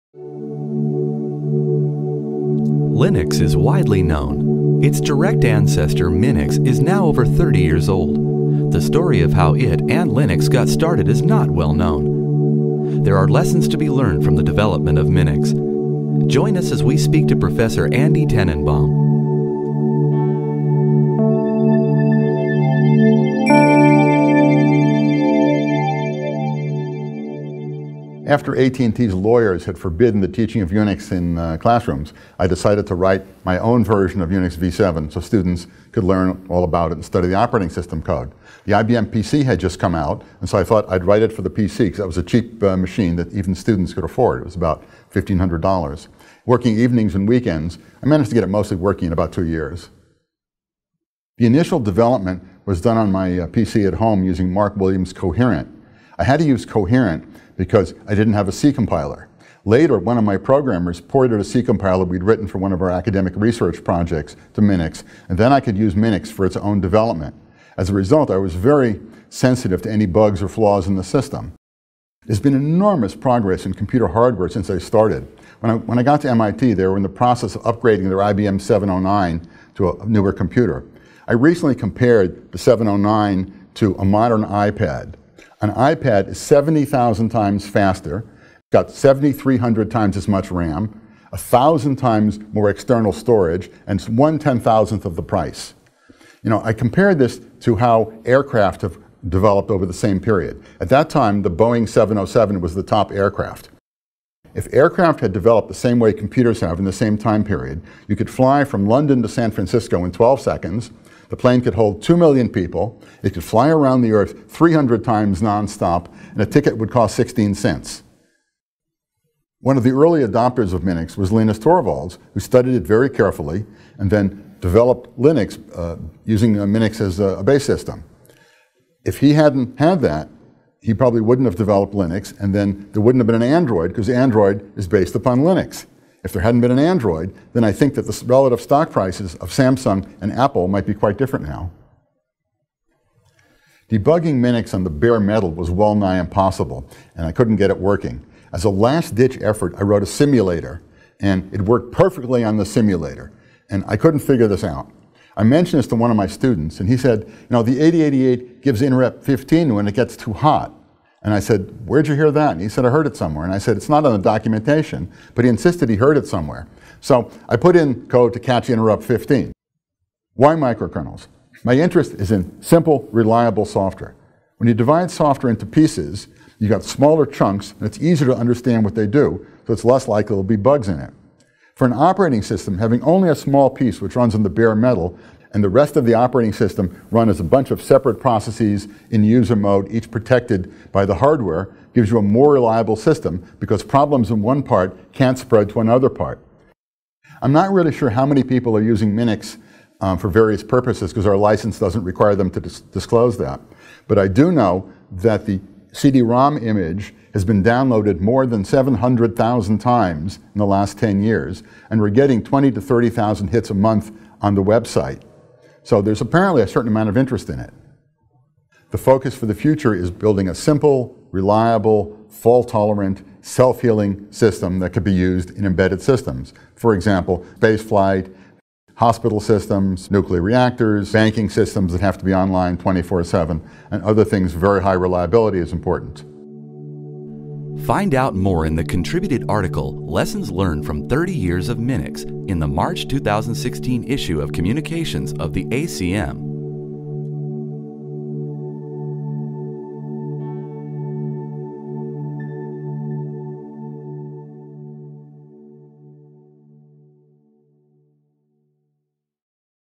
CACM Mar. 2016 - Lessons Learned from 30 Years of MINX
Andrew S. Tanenbaum, the author of the MINX operating system, discusses "Lessons Learned from 30 Years of MINIX" his Contributed Article in the March 2016 CACM.